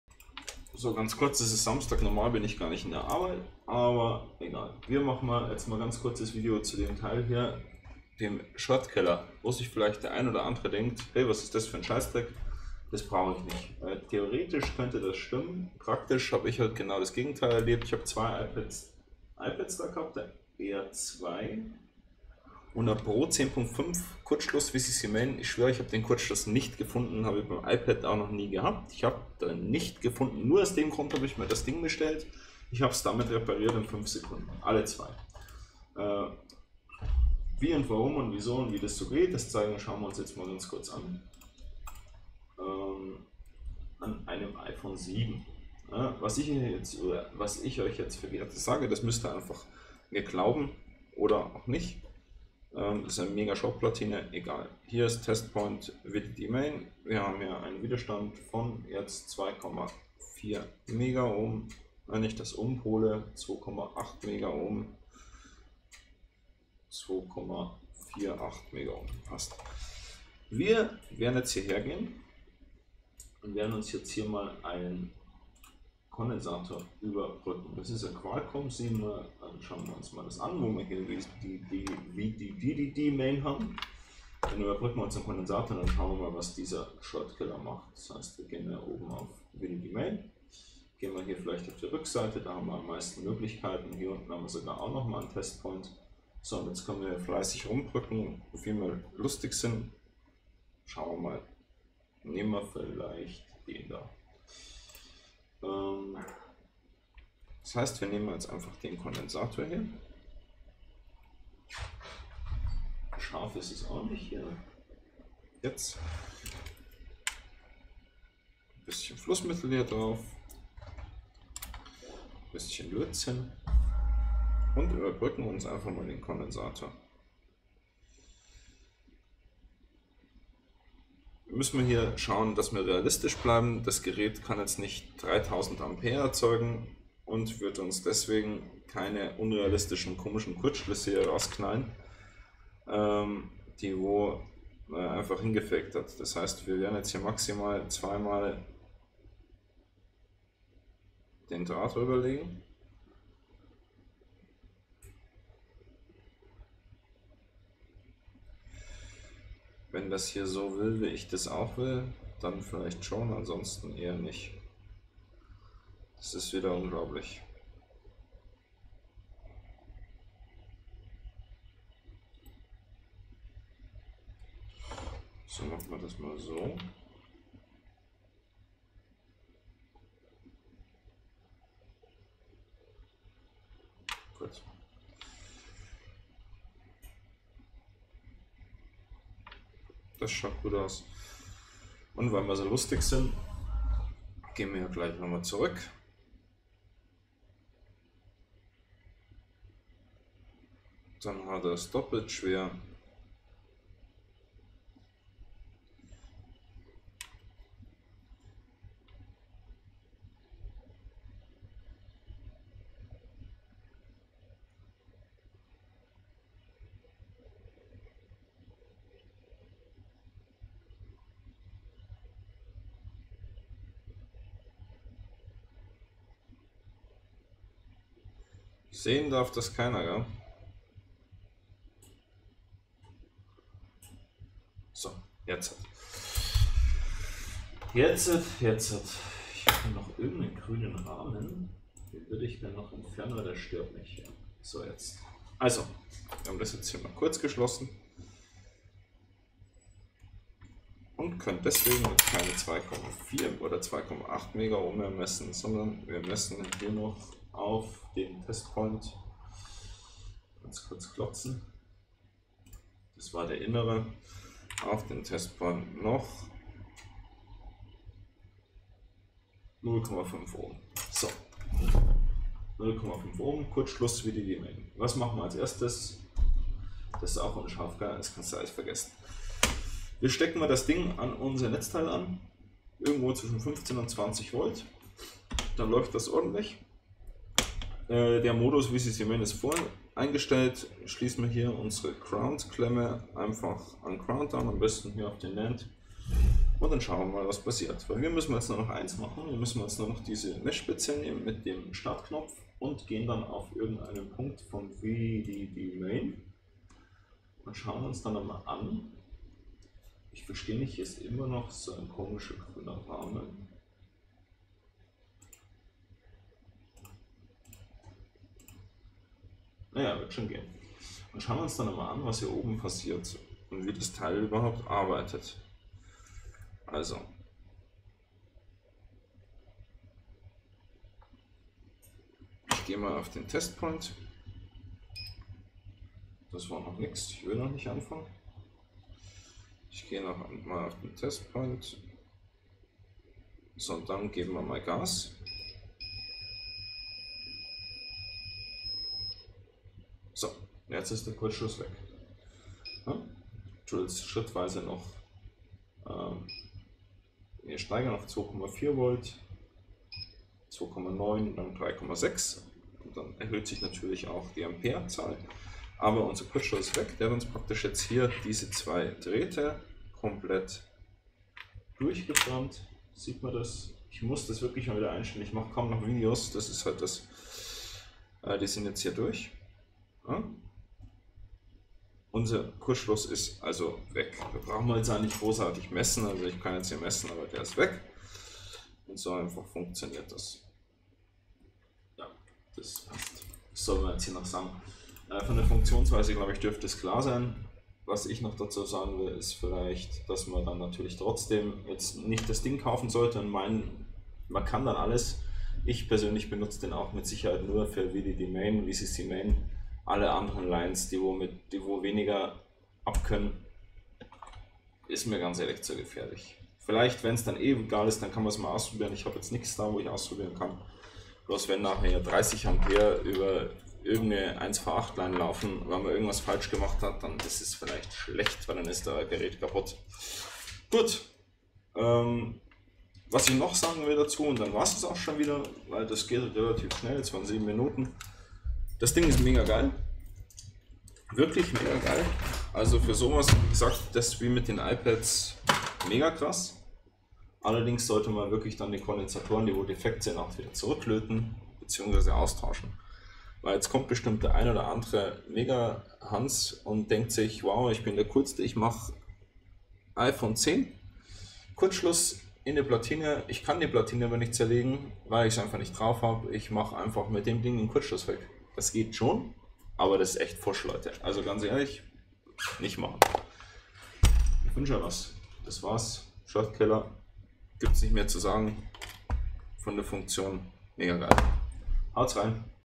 Vorstellung Shortkiller, Kurzschluss finden leicht gemacht, Kurzschluss in Sekunden entfernen
Hier zeigen wir Euch mal wie der Shortkiller an einer VCC_Main Line funktioniert.